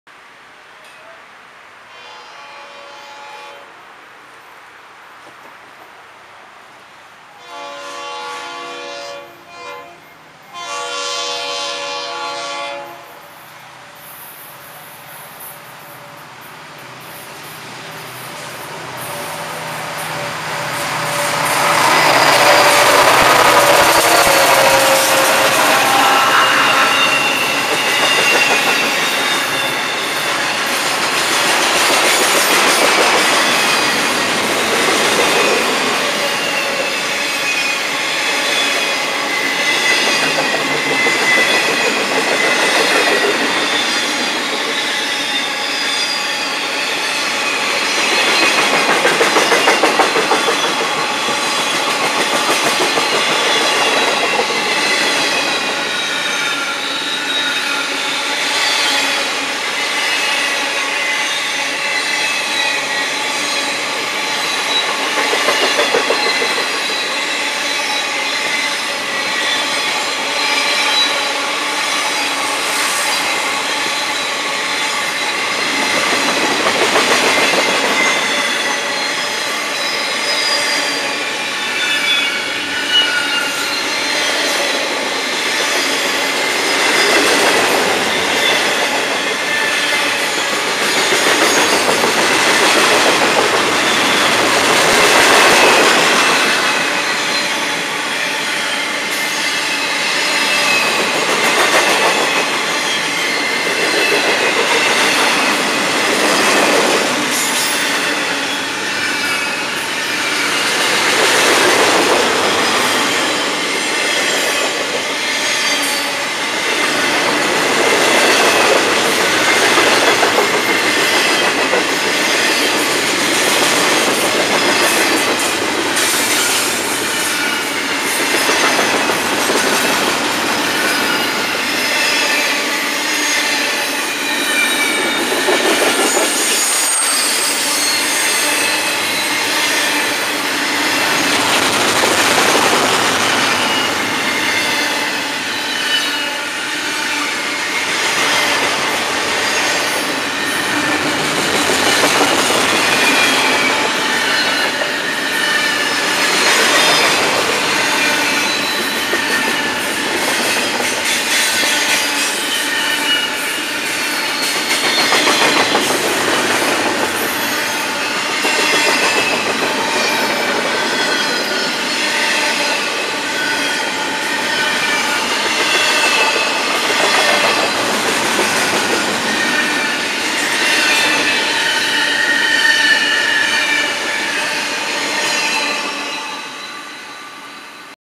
FEC Freight Train 484
FEC engines 704, 716, 429 & 431 pulling 133 cars.